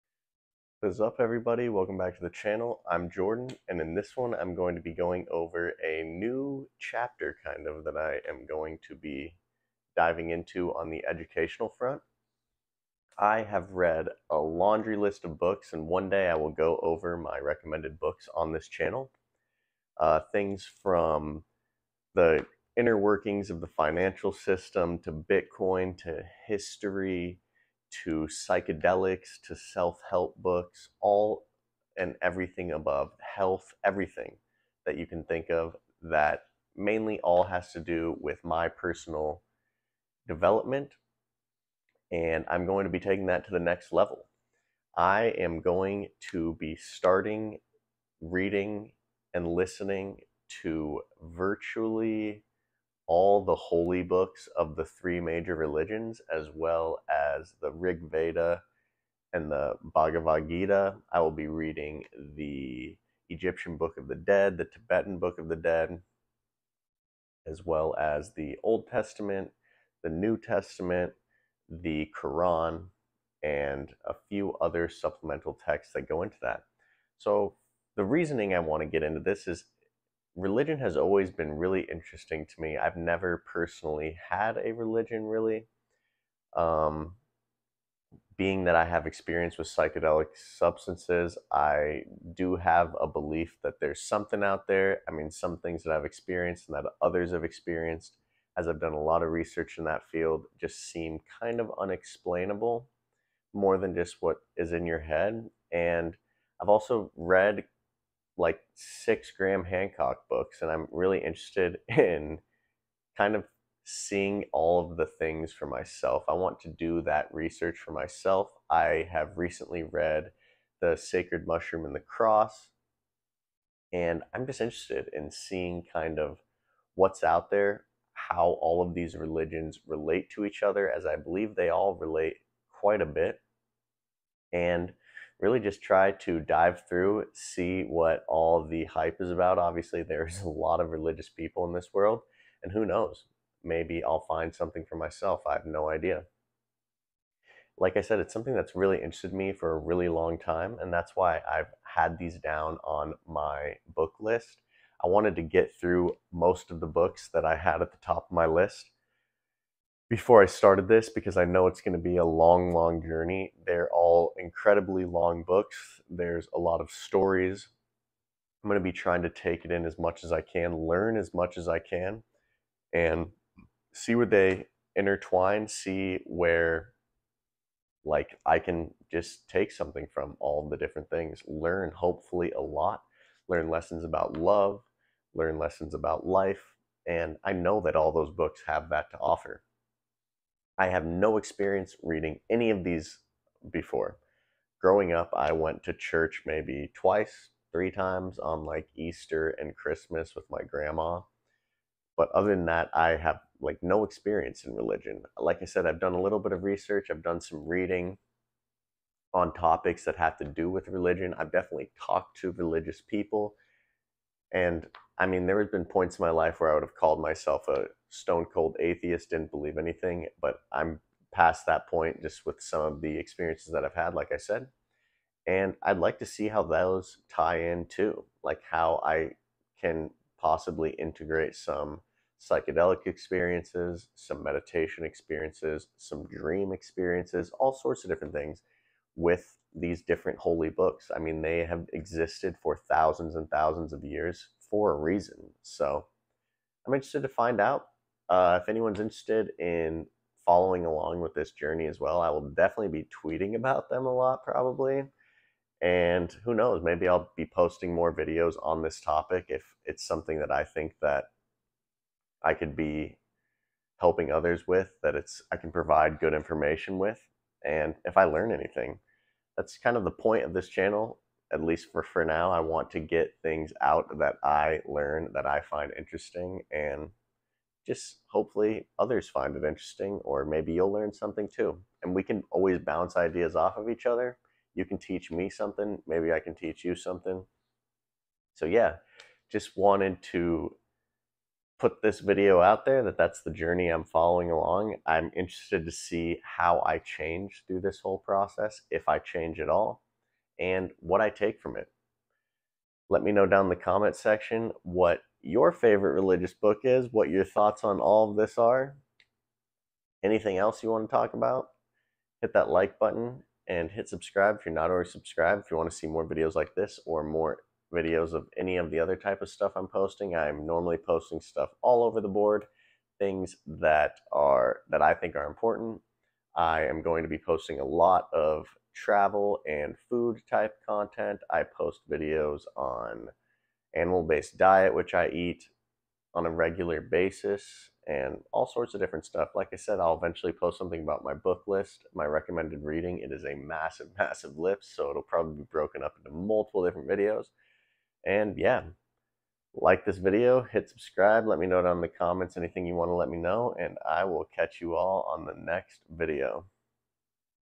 Will I Find A Religion?
In this video, I discuss my new chapter of education, religious studies.

I have never had a religion in my life, and I am long overdue for reading all the major religious texts. This is a topic that has interested me for years now and I am finally going to dive head first into the topic. If you had to put a technical term on it, I would be considered an omnist, which is "The belief that all religions contain a core recognition of the same god or gods. The belief that every entity is a god." I am excited to learn more about all these religions and possibly come out the other end with a new perspective on this whole topic.

Do you have any questions, tips, or ideas about religion?